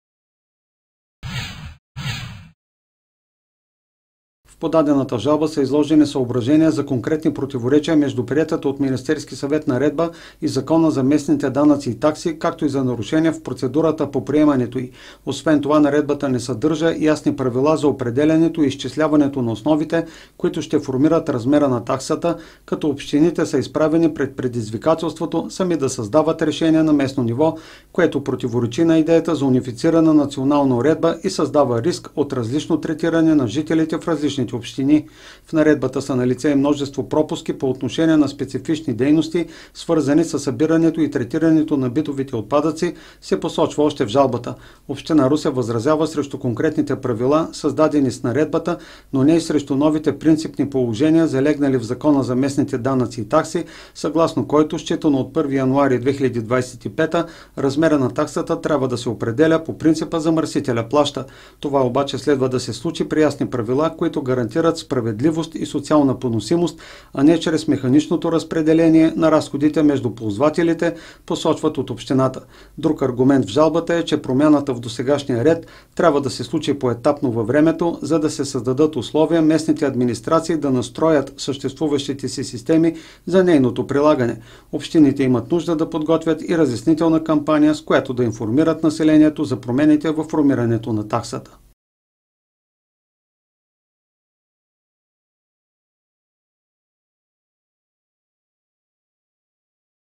Община Русе подаде жалба в съда срещу новата наредба за таксата за битовите отпадъци
Общинa Русе подаде жалба във Върховния административен съд срещу приетата от Министерския съвет през април тази година Наредба относно начина за изчисляване на размера на такса битови отпадъци. В мотивите се посочва, че нормативният акт създава множество проблеми при прилагане на уредбата и възникват предпоставки за рязко увеличаване на такса смет. В подадената жалба са изложени съображения за конкретни противоречия между приетата от Министерски съвет наредба и Закона за местните данъци и такси, както и за нарушения в процедурата по приемането ѝ.